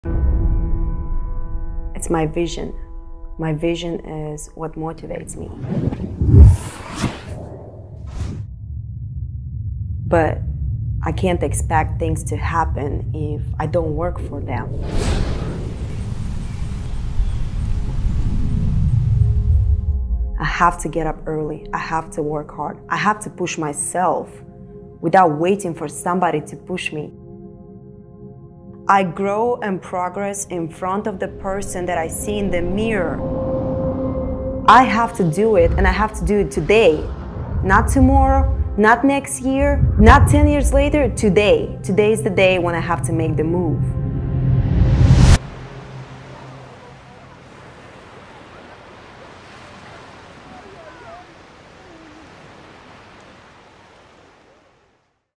Michelob Ultra Running Commercial "My Vision is what motivates me" Athlete Tatiana Verega
In life, there are no limits. Just steps.
Dream big. Never stop dreaming. Dream runner.
Brooks running shoes
Nike
Adidas

Film/Editor: Dareas A. Scott
Act by Tatiana Verega